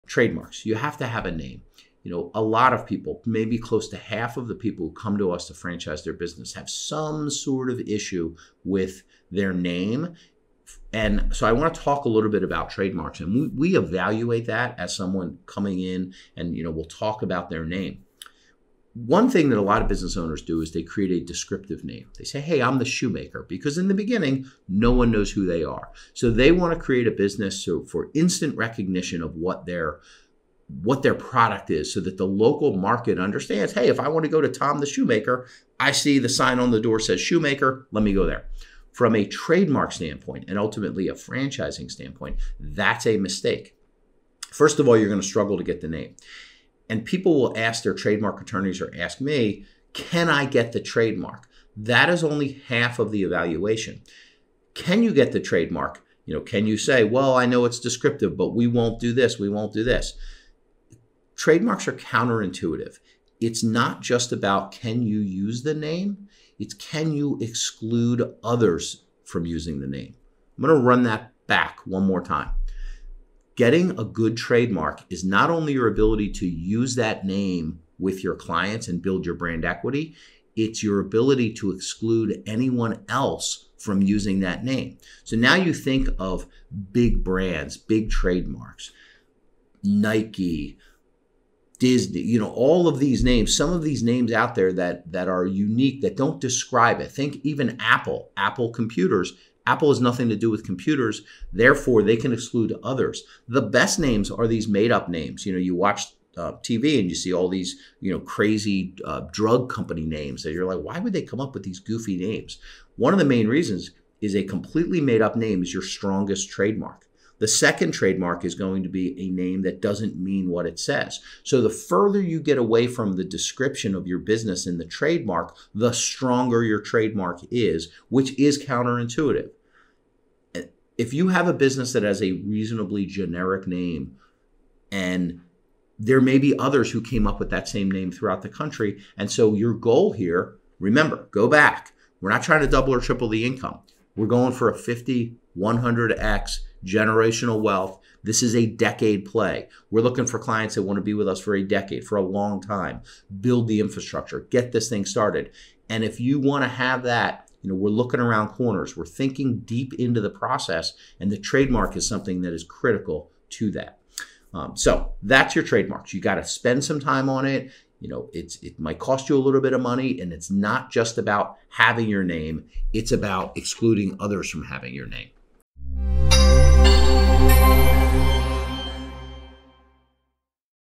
What Should Franchisors Know About Trademarks?
In this informative video, Tom Spadea addresses a common issue faced by many aspiring franchise owners: the challenges associated with business names and trademarks.

A common problem is that business owners choose overly descriptive names. While these names may offer immediate recognition in local markets, they can create significant hurdles from a trademark perspective.

Tom explores why choosing a distinctive name is crucial for branding and franchising success, and what steps business owners can take to protect their intellectual property.